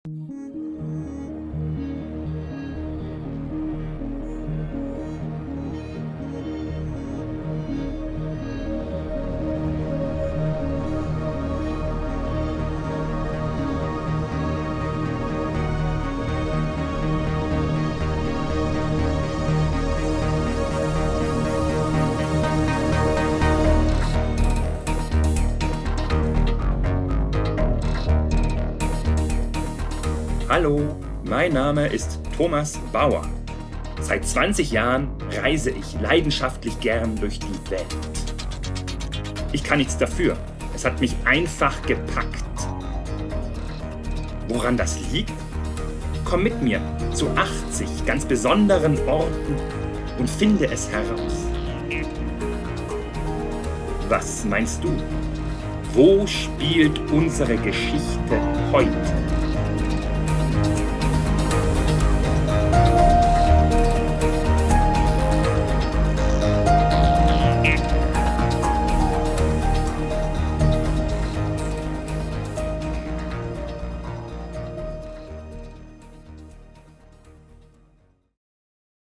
NEUGIER AUF DIE WELT Thomas Bauer - INTRO
KOMM MIT ZU 80 GANZ BESONDEREN ORTEN !!

Thomas Bauer aus München ist Abenteurer, Reisebuchautor & Fitnessfreak. Er folgte der Donau in einem Kajak zum Schwarzen Meer und dem Mississippi per Velomobil nach New Orleans, umrundete Frankreich auf einem Postrad und zog zu Fuß 2.500 Kilometer durch Europa. Im Himalaya beobachtete er einen der letzten Schneeleoparden in freier Wildbahn. Er teilte in Grönland wochenlang das Leben eines einheimischen Jägers und verbrachte mehrere Monate als Backpacker in Südamerika.

In 80 Rätseln führt uns Thomas Bauer zu besonderen Orten. Er hat sie alle besucht, teilweise länger dort gelebt. Seine Videos formieren sich zu einer höchst vergnüglichen Hymne auf das Unterwegssein. Und führen uns zu dem, was uns wirklich ausmacht.

80 Geschichten, die zeigen, wie spannend unsere Welt auch heute noch ist. Geschrieben von einem leidenschaftlichen Reisenden!

Komm mit auf die Reise ...